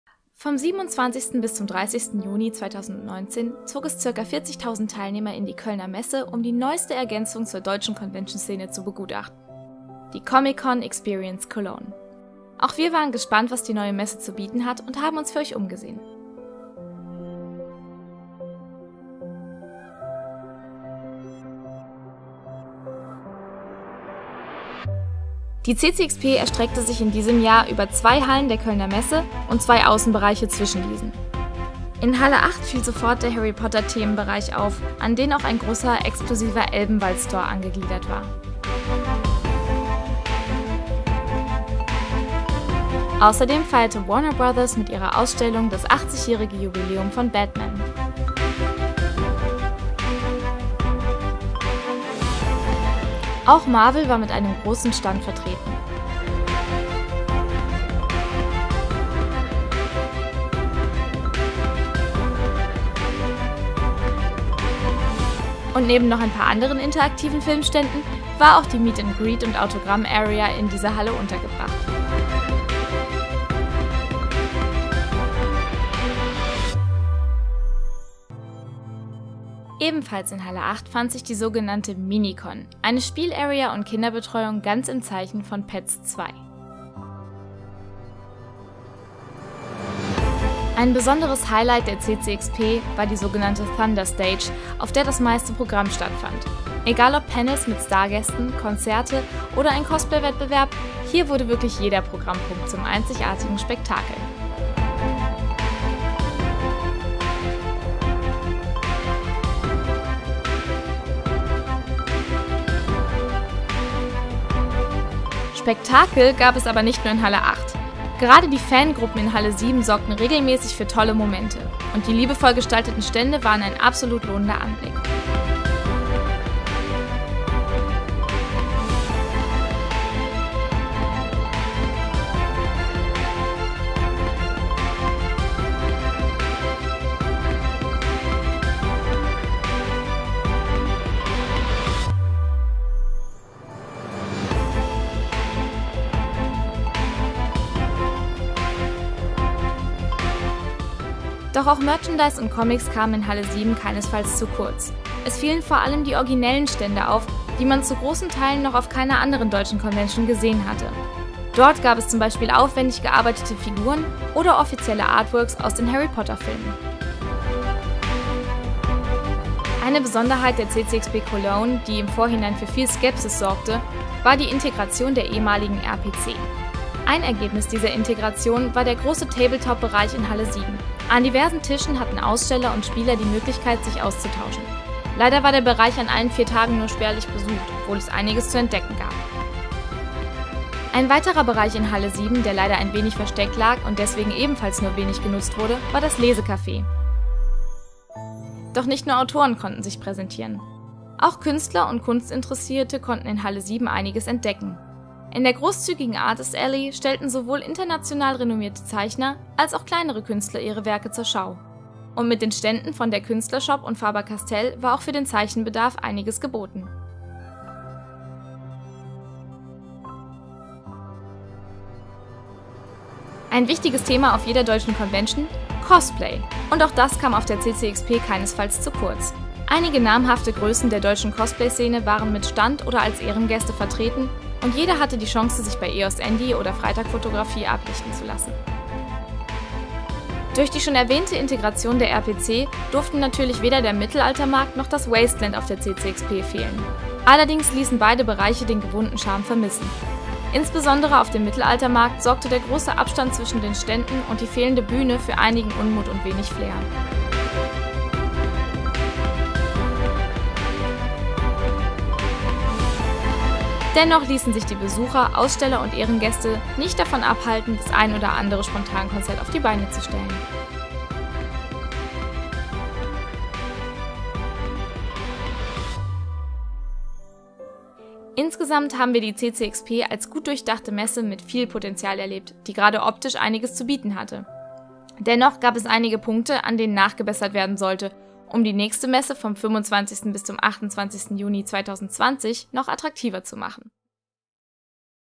[MesseRundgang] Neue Messe, neues Potential - CCXP Cologne 2019
Es hat eine halbe Ewigkeit gedauert das wissen wir, aber manchmal möchte der Alltag nicht so wie man selbst.
Aber JETZT ist es endlich so weit: Unser Rundgang zur CCXP Cologne ist fertig und es gab wirklich einiges zu sehen! Viele von euch waren vielleicht nicht auf der Messe, die hohen Preise und die Tatsache, dass die Con zum ersten Mal stattfand, also wollten wir euch einen bestmöglichen Eindruck verschaffen!
Hoffentlich hat das geklappt und euch gefällt das Video!

Zum Video:
Kamera: Thomas & Michael
Schnitt, Ton und Voiceover: Laura
Ihr seid im Video und wollt verlinkt werden? Meldet euch einfach bei uns!

Unterstütze uns mit deinem Kauf bei Amazon!

Wie könnt ihr MesseHunter erreichen?